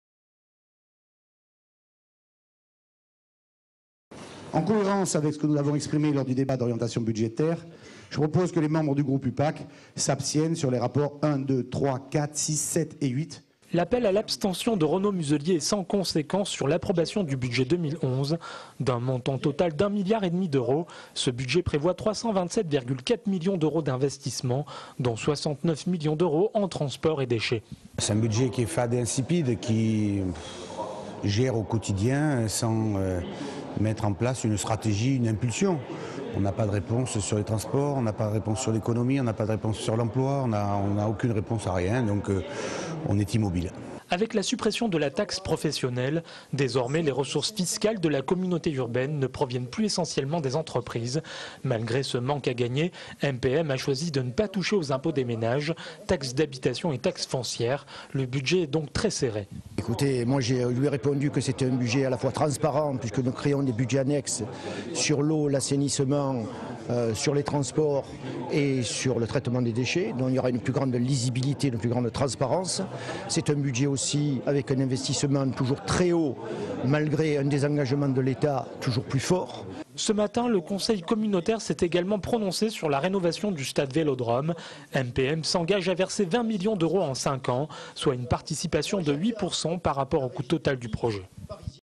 MPM : le budget a été adopté
MPM a voté son budget pour 2011, qui est sensiblement le même que l'an passé avec une bonne nouvelle: la fiscalité n'augmente pas!
DROITS RESERVES. Pour conserver ce reportage